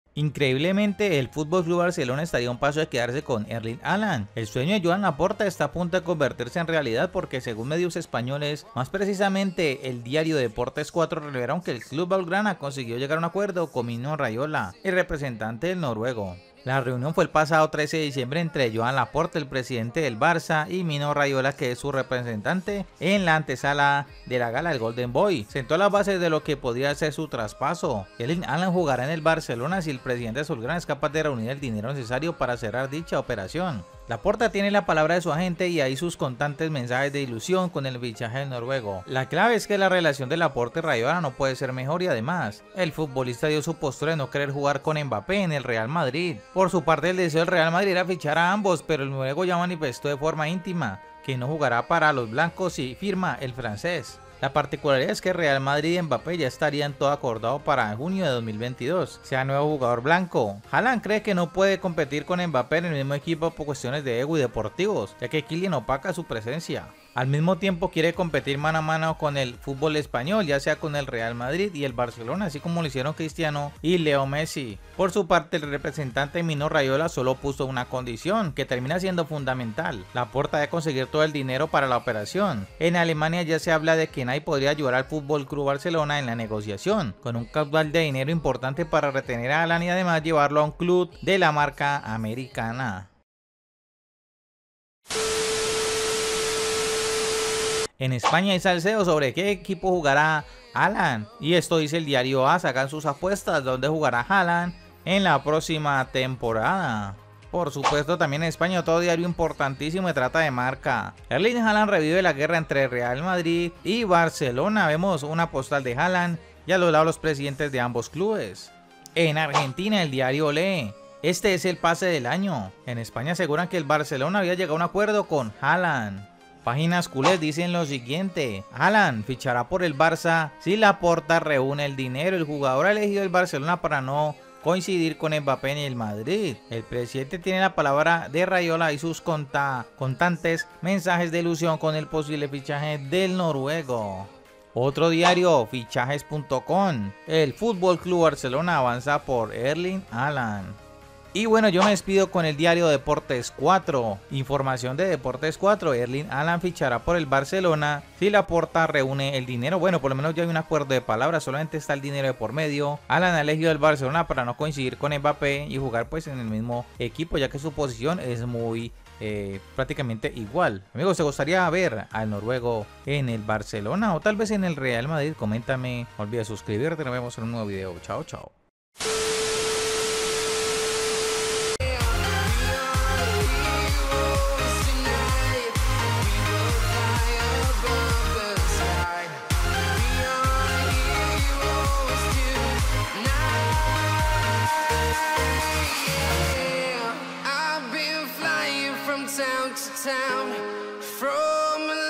🔥Erling Haaland al Barcelona 2024 Laporta y Raiola ya saben su Futuro
Increíblemente, el FC Barcelona estaría a un paso de quedarse con Erling Haaland. El sueño de Joan Laporta está a punto de convertirse en realidad porque según medios españoles, más precisamente Deportes Cuatro, revelaron que el club blaugrana consiguió llegar a un acuerdo con Mino Raiola, el representante del noruego.

"La reunión del pasado 13 de diciembre entre Joan Laporta y Mino Raiola en la antesala de la gala Golden Boy sentó las bases de lo que podría ser el traspaso. Erling Haaland jugará en el Barcelona si el presidente azulgrana es capaz de reunir el dinero necesario para cerrar la operación. Laporta tiene la palabra del agente y de ahí sus contantes mensajes de ilusión con el fichaje del noruego", reveló Cuatro.

La clave es que la relación de Laporta y Raiola no puede ser mejor y además, el futbolista dio su postura: no quiere jugar con Kylian Mbappé. El deseo del Real Madrid era fichar a ambos, pero el noruego ya manifestó de forma íntima, que no jugará para los blancos si firma el francés.

La particularidad es que entre Real Madrid y Mbappé ya estaría todo acordado para que en junio del 2022, sea nuevo jugador blanco. Haaland cree que no puede compartir con Mbappé el mismo equipo por cuestiones de ego y deportivas, ya que Kylian opacaría su presencia.

Al mismo tiempo, quiere competir mano a mano en el fútbol español con Mbappé y por eso dio luz verde al fichaje por el FC Barcelona. Mino Raiola solo puso una condición, que termina siendo fundamental: Laporta debe conseguir todo el dinero para la operación.

En Alemania ya se habla de que Nike, podría ayudar al FC Barcelona en la negociación con un caudal de dinero importante para retener a Haaland y además, llevarlo a un club de la marca americana.

🔔RECIBE NOTIFICACIONES CADA VEZ QUE PUBLICO UN NUEVO VÍDEO:
2. Dale Check a: "Recibir todas las notificaciones de este canal"
3. Dale a guardar